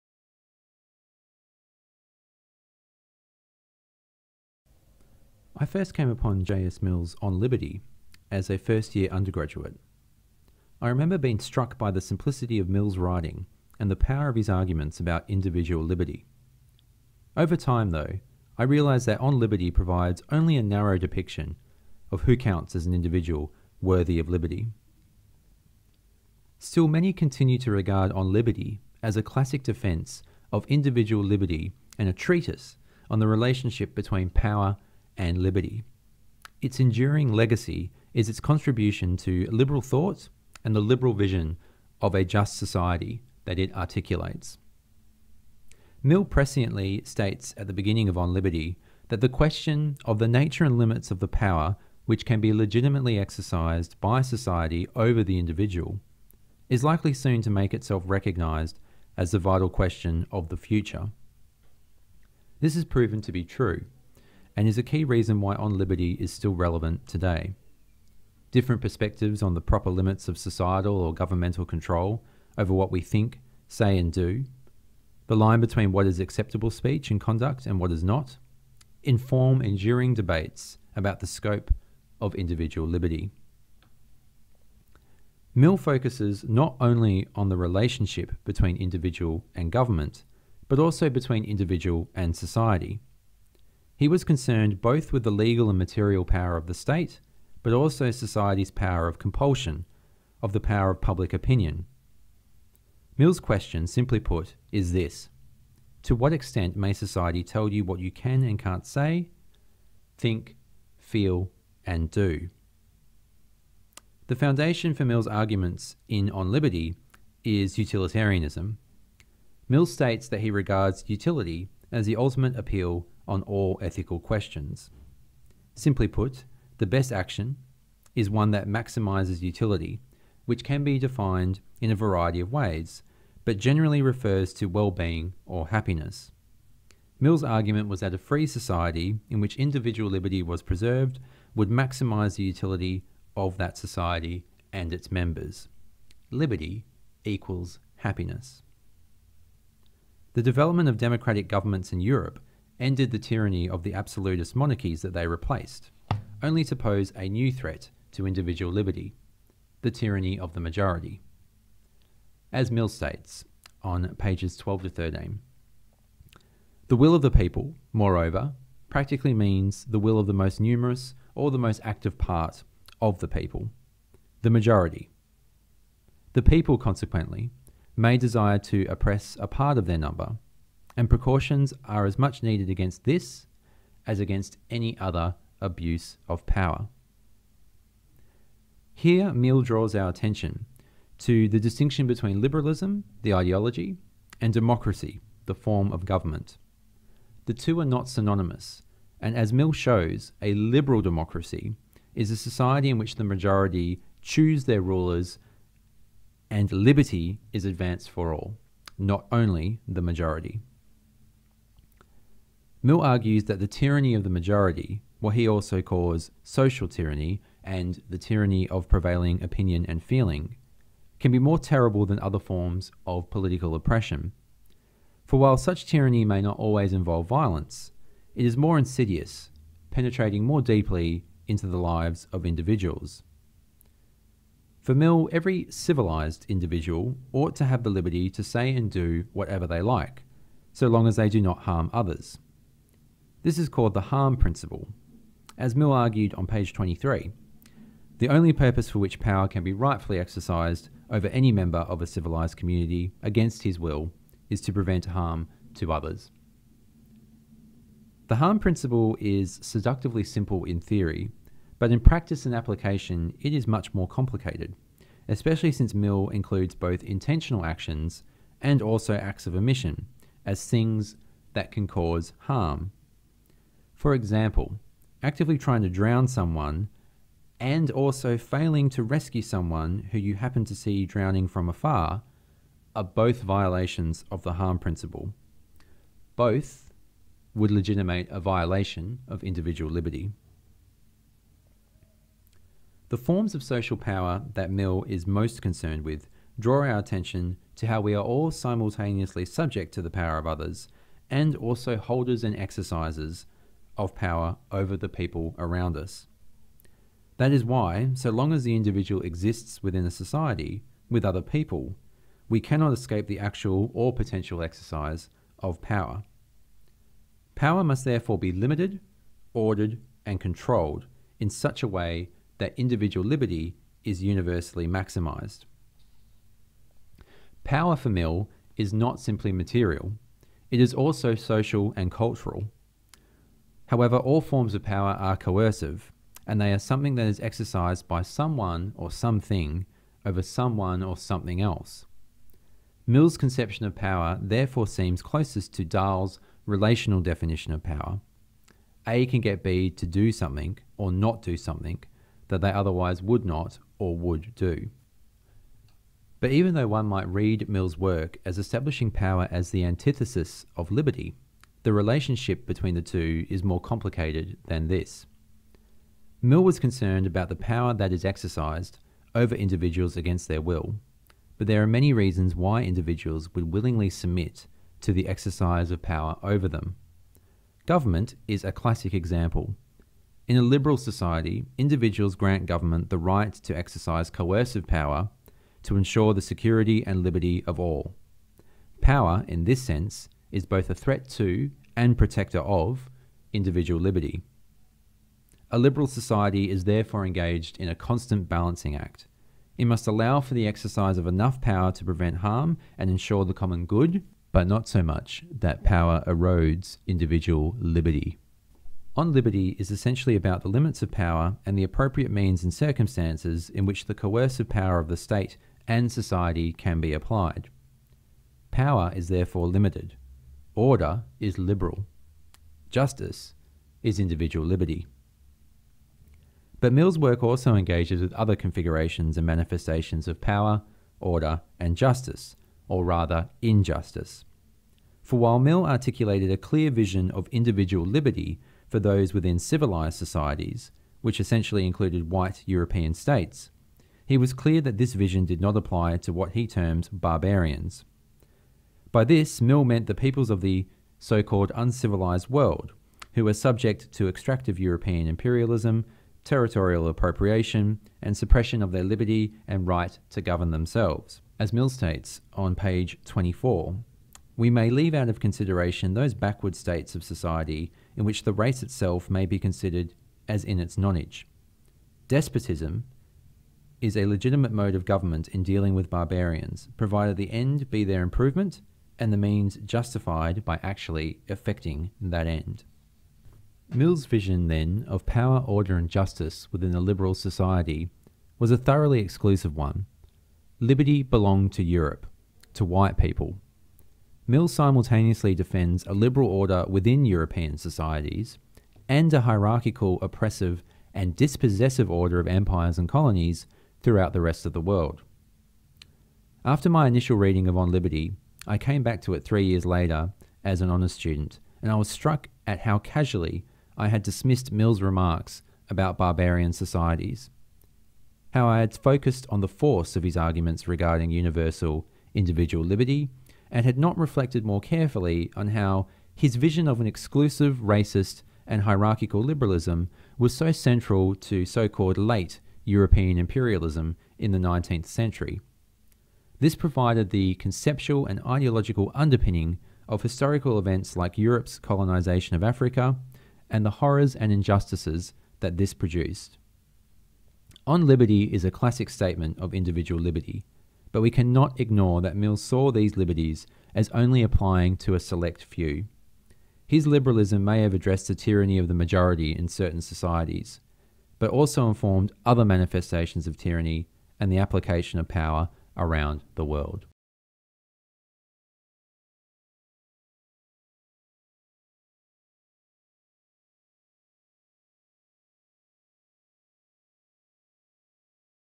John Stuart Mill
VideoScribe animation that introduces the work of John Stuart Mill in relationship to power, politics and International Relations. Scripted and read by Dr William Clapton.